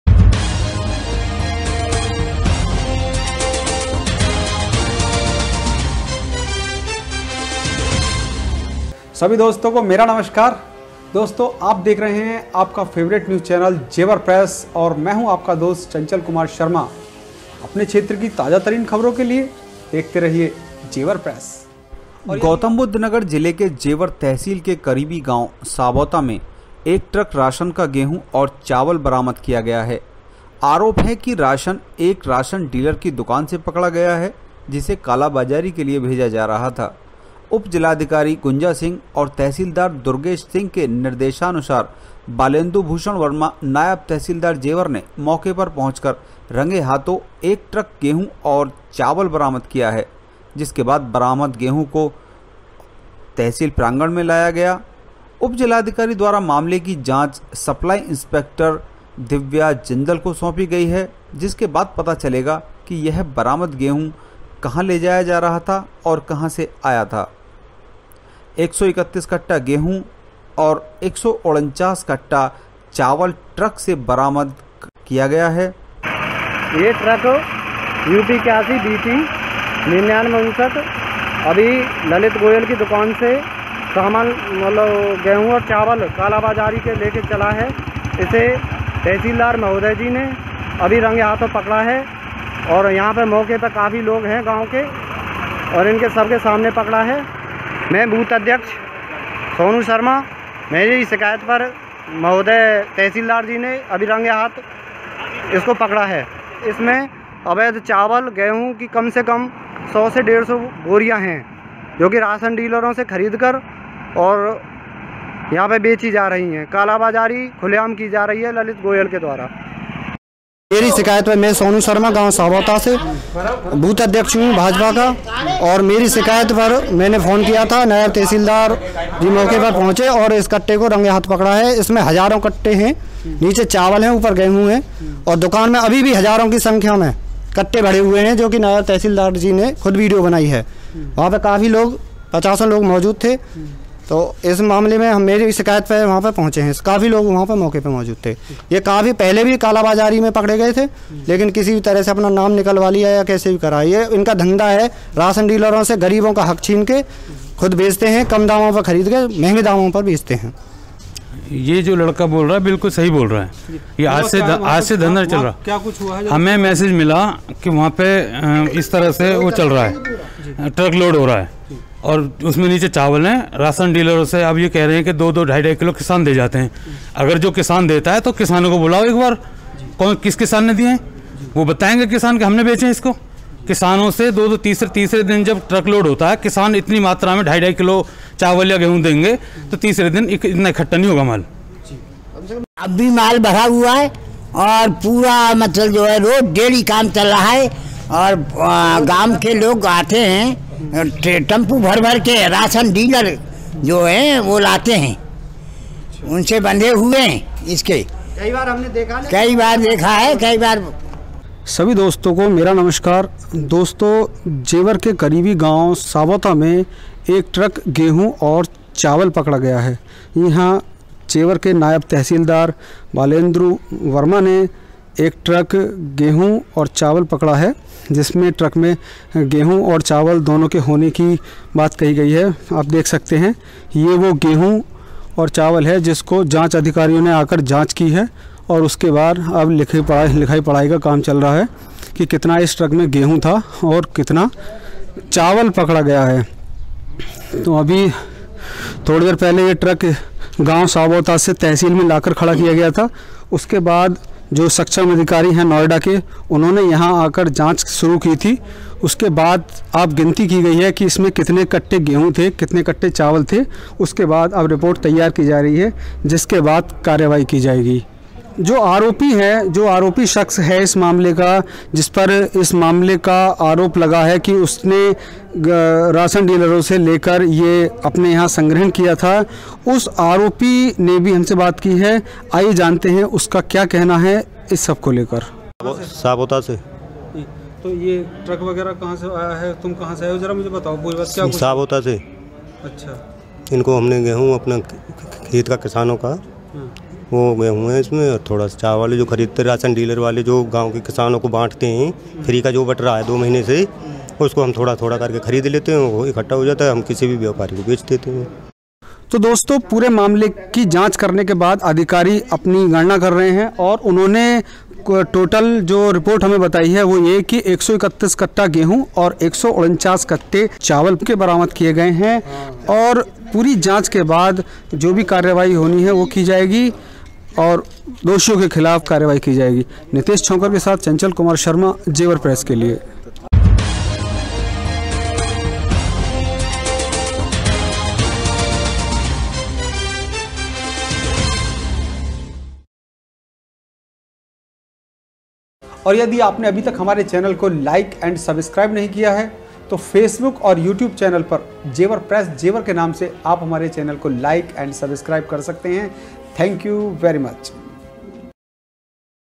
JEWAR PRESS- तहसील प्रशासन ने राशन की कालाबाजारी पकड़ी, गेहूं और चावल के लगभग 270 बोरे बरामद किए ,
जेवर में राशन की कालाबाजारी पकड़ी,  गेहूं और चावल के लगभग 270  बोरे बरामद  किए , जेवर तहसील प्रशासन की कालाबाजारी के खिलाफ बड़ी कार्रवाई,